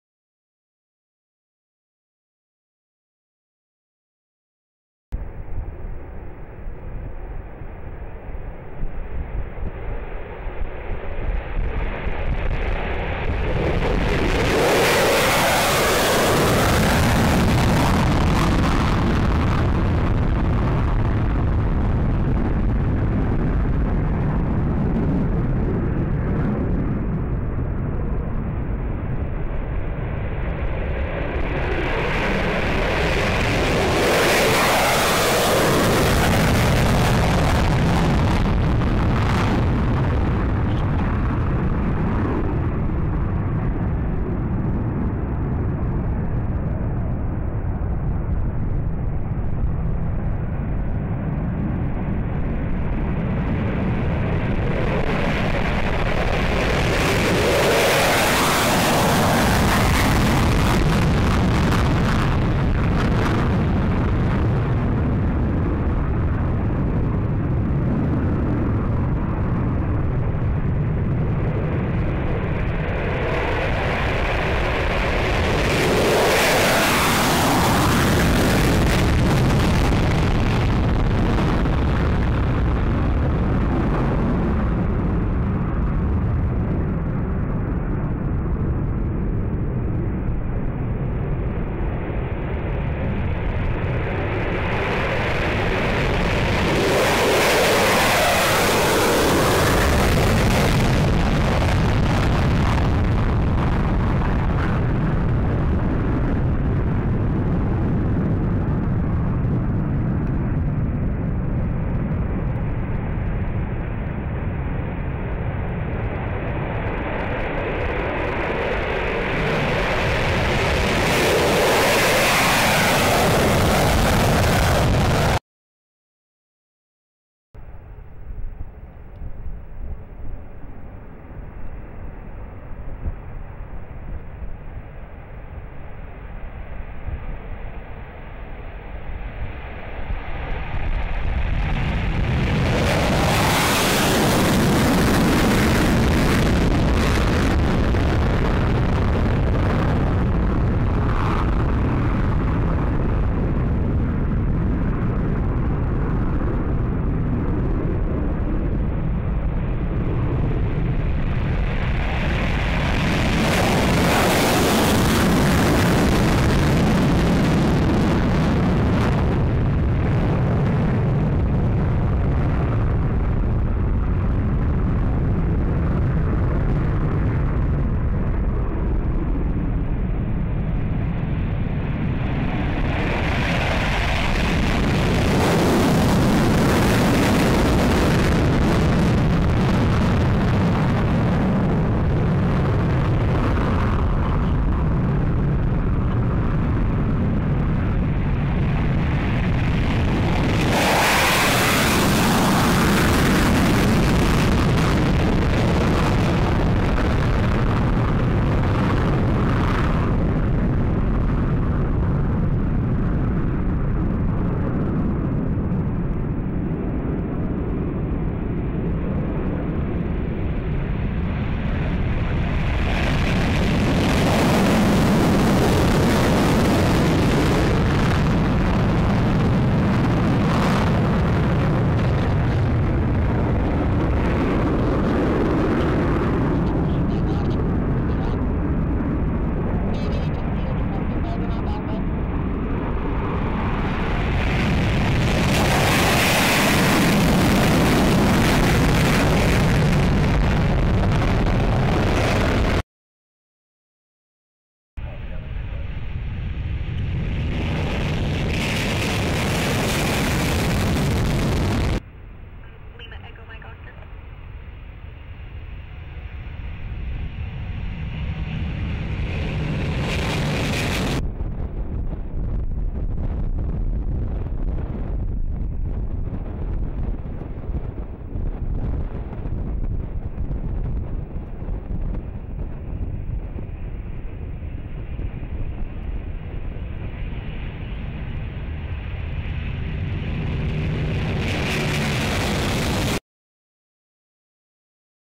F 22, F 15, KC 10 take offs in Southwest Asia
Uncut take-offs of F-22, F-15, & KC-10s out of the 380th Air Expeditionary Wing in Southwest Asia. Unit hometowns: (F-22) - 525 FS - Joint Base Elmendorf-Richardson, Alaska, (F-15) - 4 FW - Seymour Johnson AFB, NC, and (KC-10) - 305 AMW - Joint Base McGuire-Dix-Lakehurst, NJ.
This work by TSgt Colleen Urban